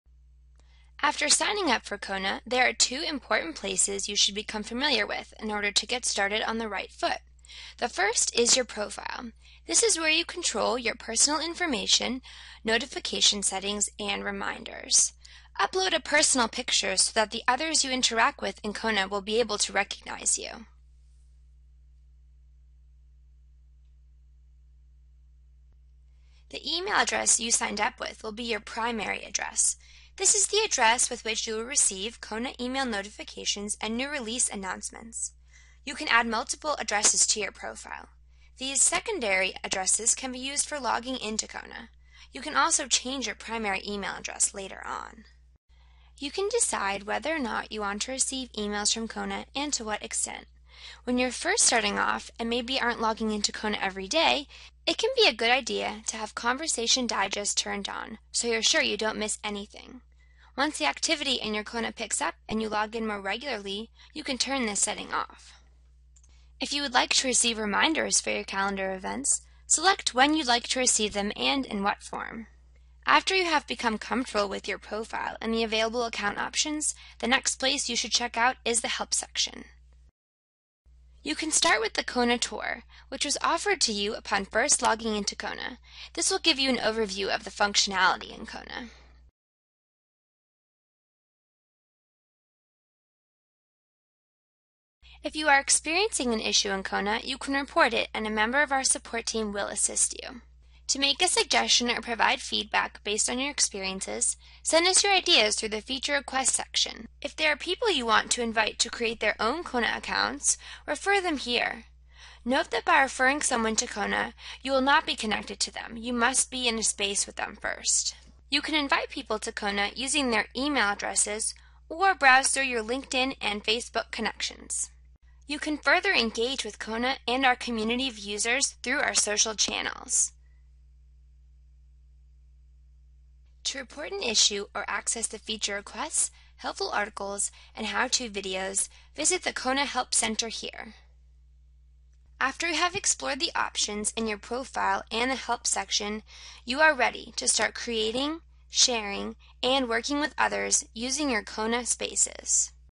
Getting Started: Your Profile & the Kona Community
This video gives you an overview of your profile and help center in Kona.